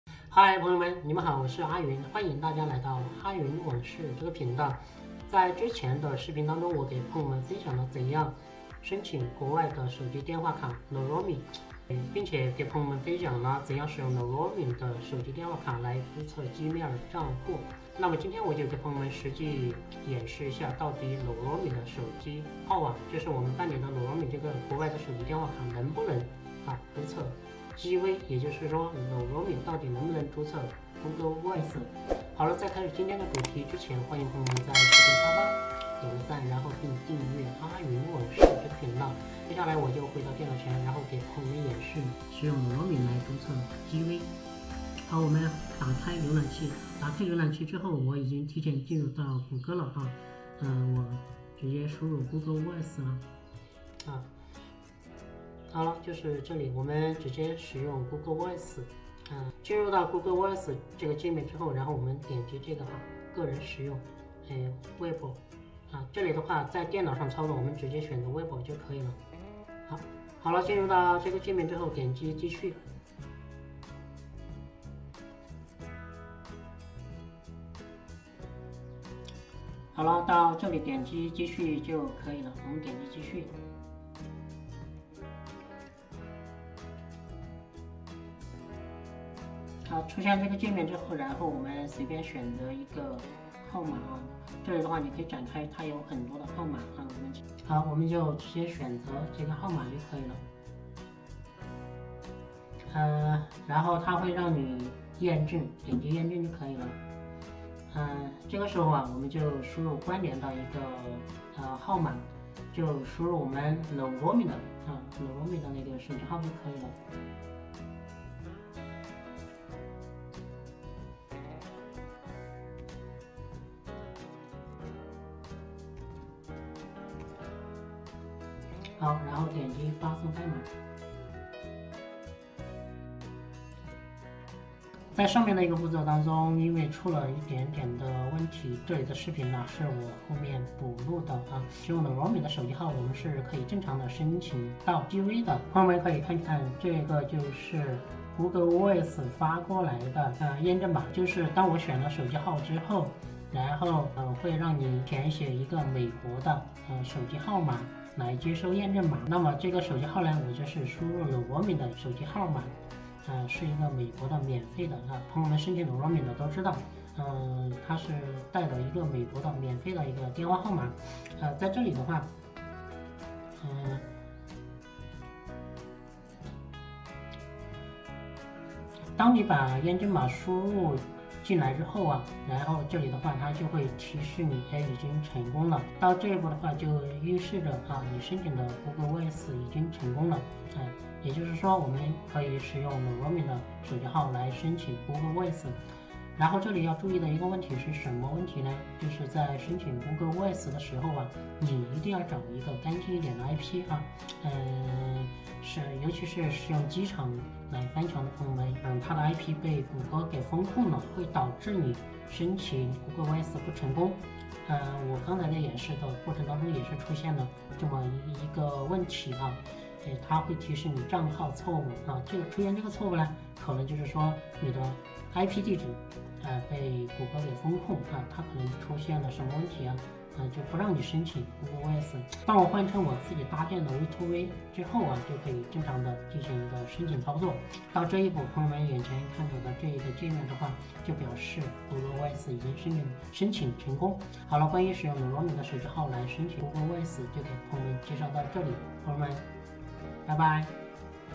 How to register with google voice using Knowroaming sim card?，Knowroaming注册Google Voice【阿雲網事】#31
How to register with google voice using Knowroaming sim card ?

怎样使用申请的Knowroaming手机号注册Google Voice？实践证明，使用Knowroaming的手机号是可以注册Google Voice。
国内注册Google Voice通常需要翻墙，可自己搭建V2ray；也可以使用VPN，下面的VPN都是付费VPN，松鼠VPN是去中心化的VPN，邀请码可以免费使用3天。

几款好用的VPN（如果购买建议先付费1个月，体验一下连接是否稳定）：